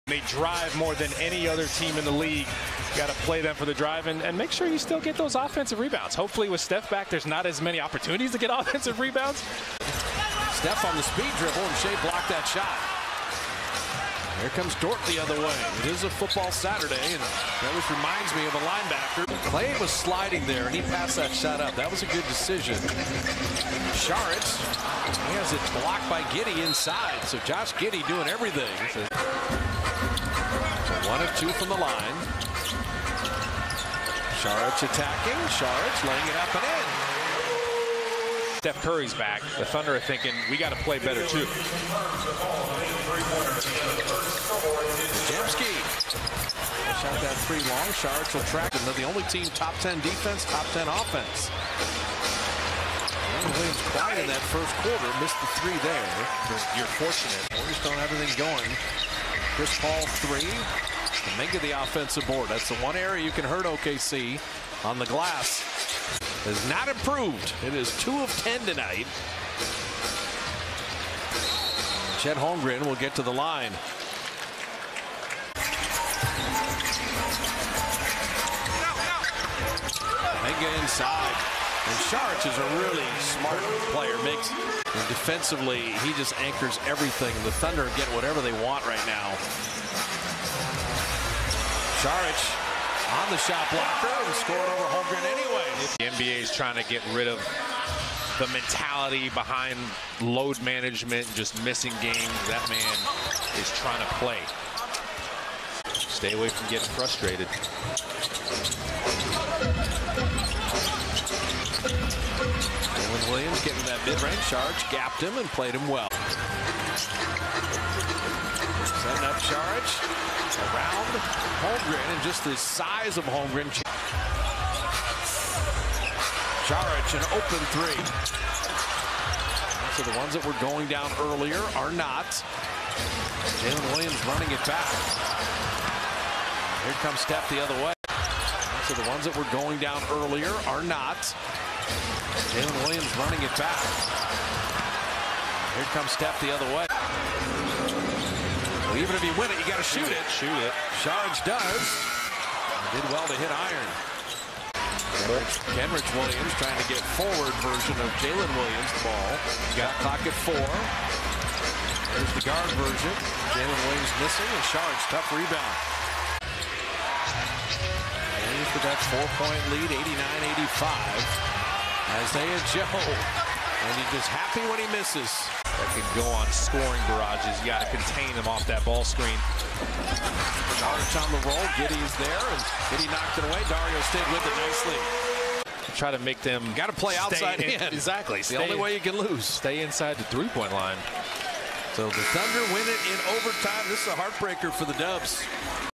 Dario Saric 6 pts 8 reb 1 ast vs Oklahoma City Thunder | 2023-11-18
Dario Saric Highlights from Oklahoma City Thunder vs Golden State Warriors on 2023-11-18.

Player Stats:
18:22 mins
6 Points
8 Rebounds (2 Offensive)
1 Assists
0 Steals
0 Blocks
1 Turnovers
3/9 Field Goal
0/0 Free Throw